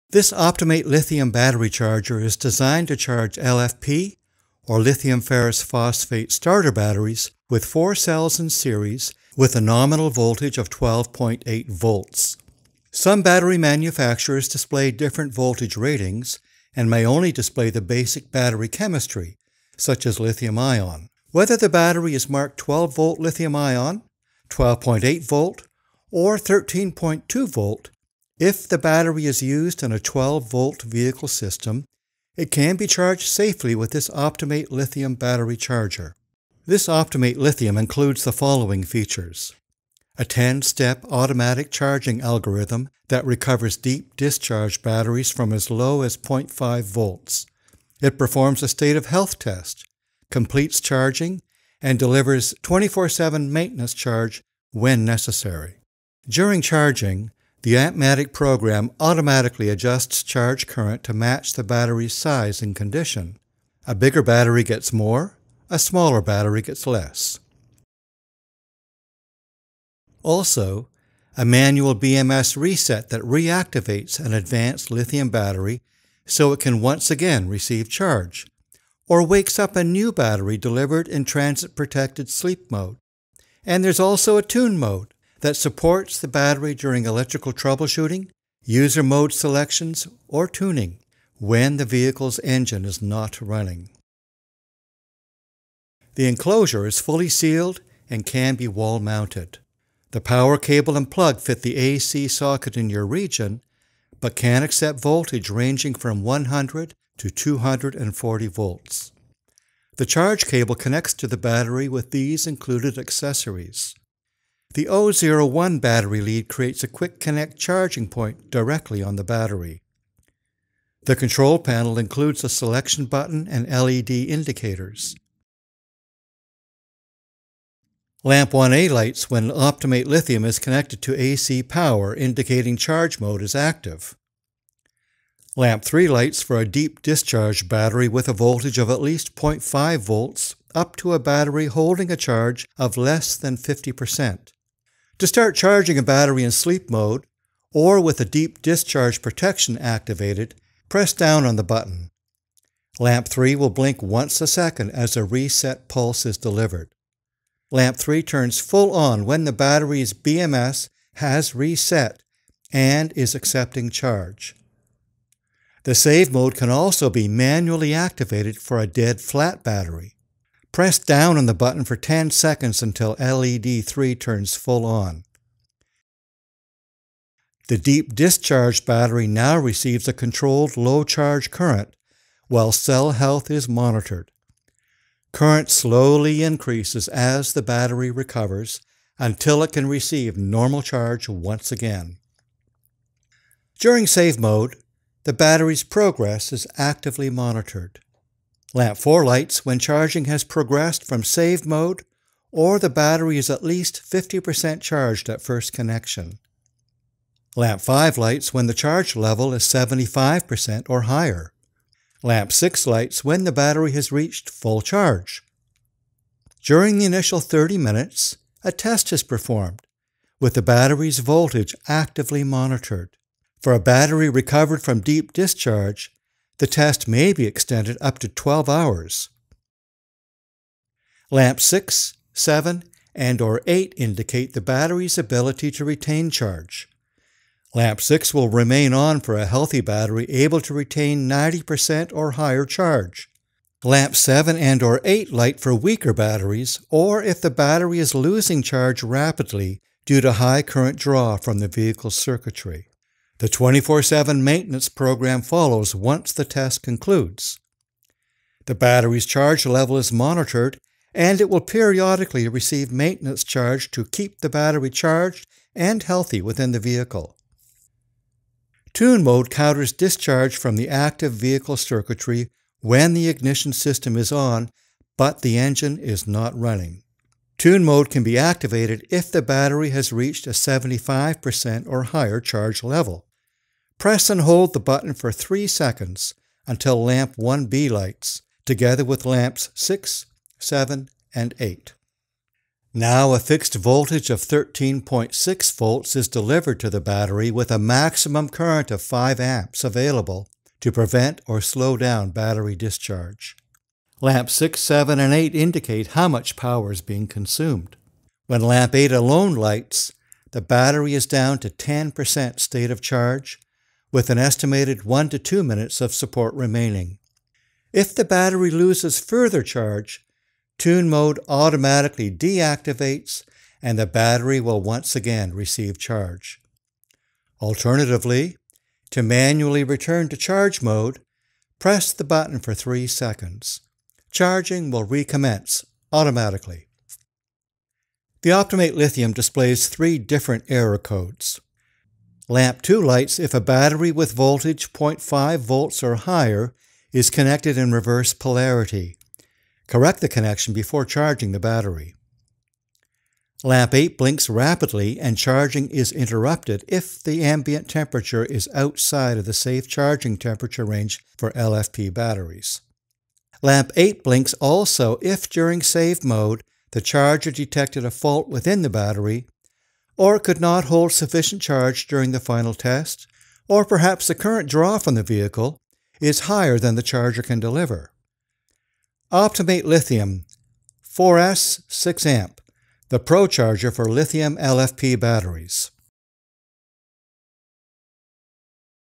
Fast charger for Li-ion battery | OptiMate Lithium 4s 6A
The OptiMate Lithium 4s 6A is our ADVANCED fast charger for the lithium-ion battery in your performance/race motorcycle, car or craft. This video also explains how to reset BMS protection; and explains how CHARGE mode & TUNE mode works.

The OptiMate Lithium 4s 6A battery charger is designed to charge LFP or Lithium Ferrous Phosphate starter batteries with 4 cells in series, with a nominal voltage of 12.8V.
OptiMate Lithium 4s 6A, the PRO charger for Lithium LFP batteries.

Video contents:
00:00 – This OptiMate Lithium battery charger
00:36 – Included features
01:11 – Manual BMS reset
01:36 – Overview of product hardware
02:05 – CHARGE mode
04:29 – TUNE mode
05:39 – ERROR codes
06:19 – OptiMate Lithium 4s 6A